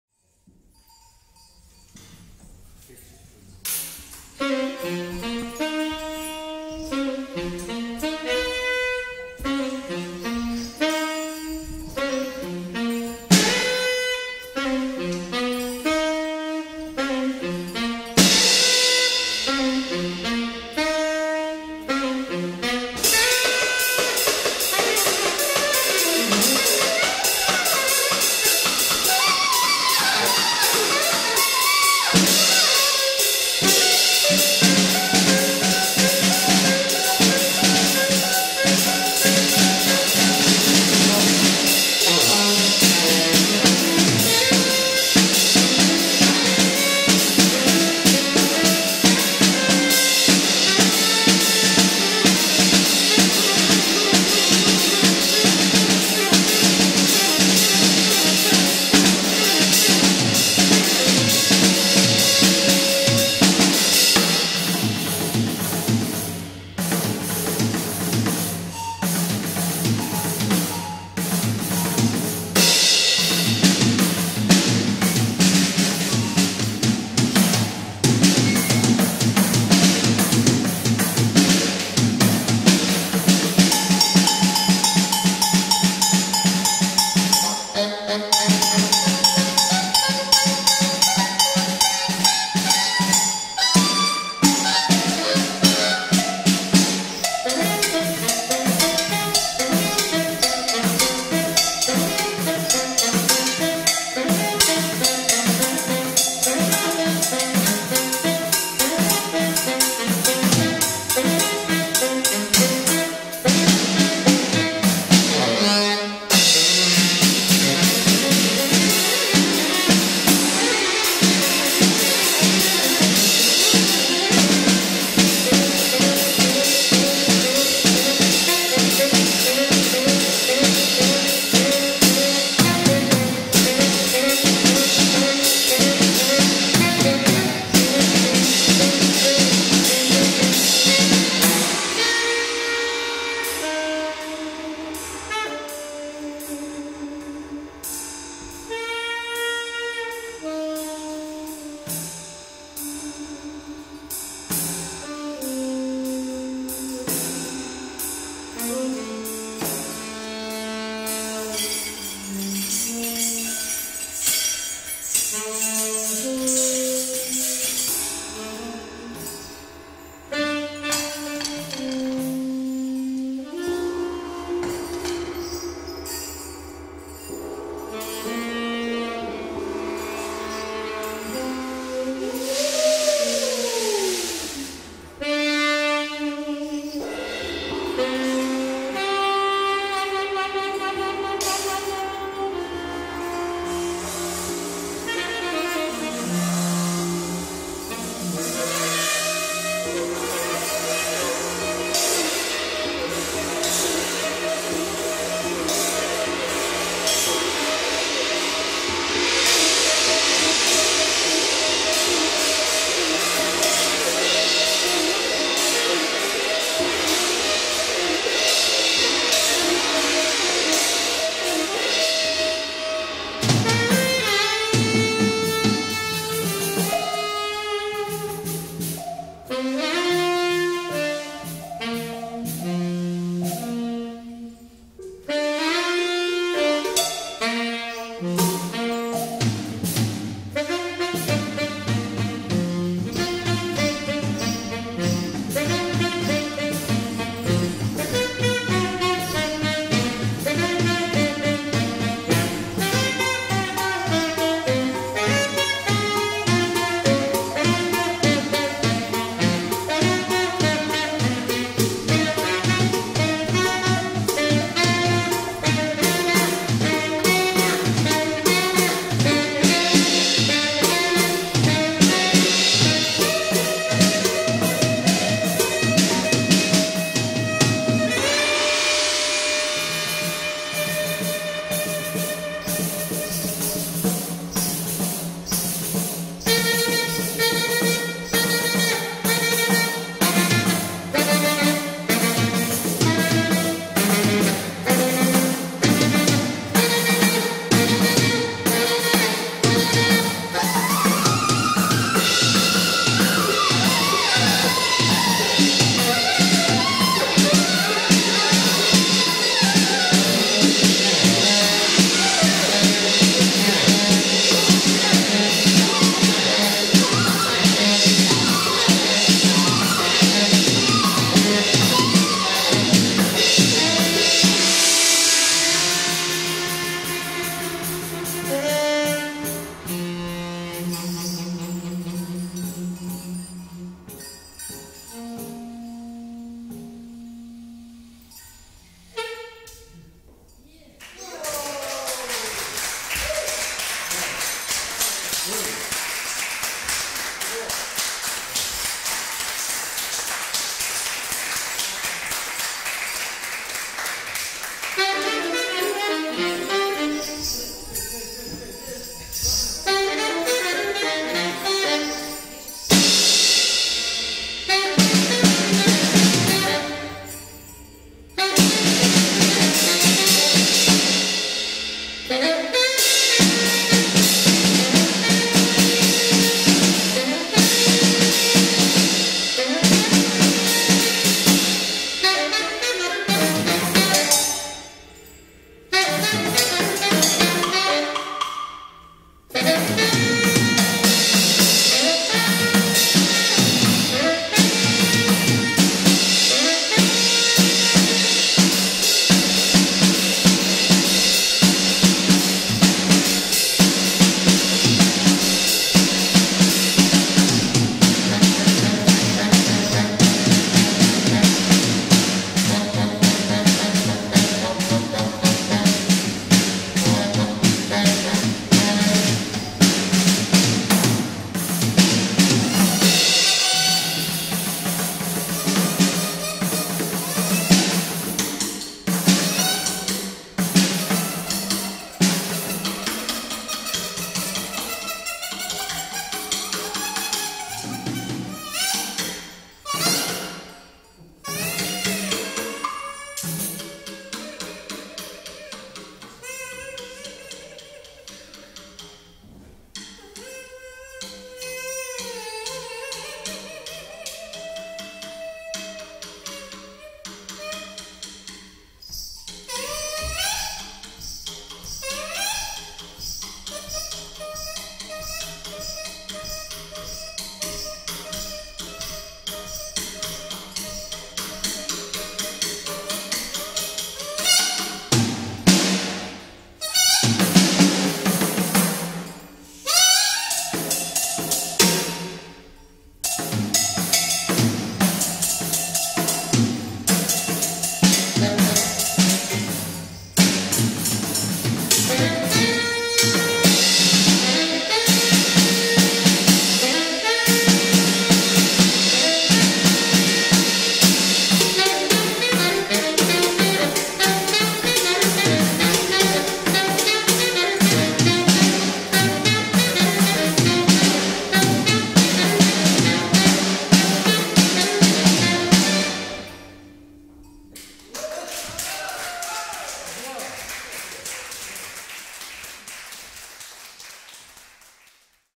Massey & Aalberts at City Art Rotterdam part 2
Zane Wayne Massey & Coen Aalberts performing at City Art Rotterdam

Video & Sound recorded by Herman Koekkoek